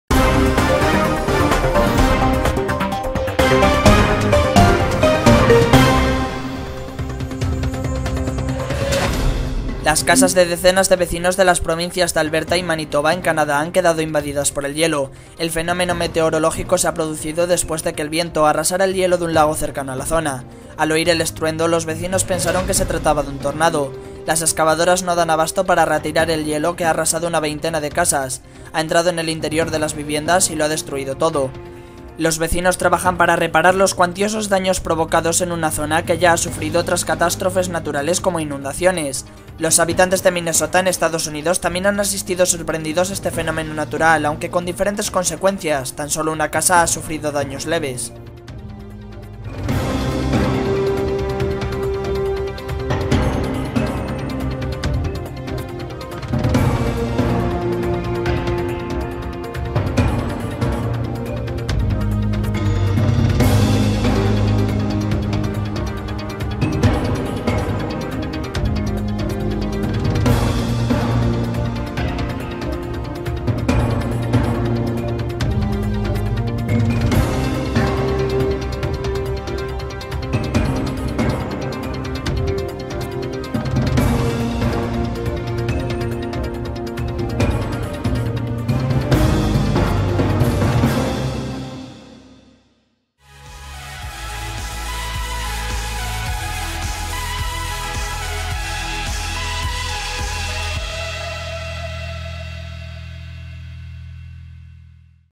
UN TSUNAMI DE HIELO ARRASA CANADA - 13/5/2013
Las casas de decenas de vecinos de las provincias de Alberta y Manitoba en Canadá han quedado invadidas por el hielo. El fenómeno meteorológico se ha producido después de que el viento arrasara el hielo de un lago cercano a la zona. Al oír el estruendo los vecinos pensaron que se trataba de un tornado.

Las excavadoras no dan abasto para retirar el hielo que ha arrasado una veintena de casas. Ha entrado en el interior de las viviendas y lo ha destruido todo.

Los vecinos trabajan para reparar los cuantiosos daños provocados en una zona que ya ha sufrido otras catástrofes naturales como inundaciones.

Los habitantes de Minnesota en Estados Unidos también han asistido sorprendidos a este fenómeno natural aunque con diferentes consecuencias. Tan solo una casa ha sufrido daños leves.

El misterioso espectáculo ocurre al pasar de temperaturas bajo cero, que permiten la congelación del agua, a temperaturas por encima de los 25 grados, que provocan el deshielo. El fuerte viento arrastra esos fragmentos helados y se produce este curioso efecto.